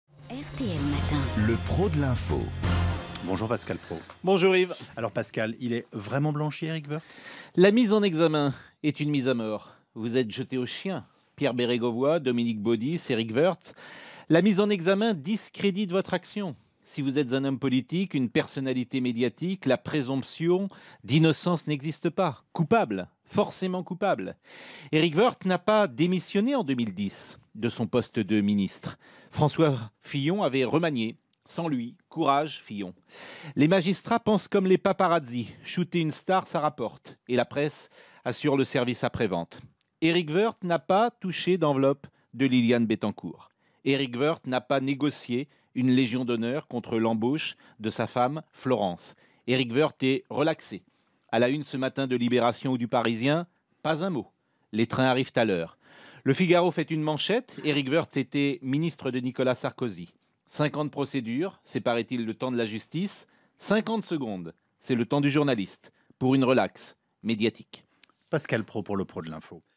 Éric Woerth : "La mise en examen est une mise à mort", dit Pascal Praud - RTL - RTL
ÉDITO - Éric Woerth vient d'être relaxé par le tribunal correctionnel de Bordeaux ce jeudi 28 mai. Jugé en mars pour "trafic d'affluence", il a toujours nié les faits.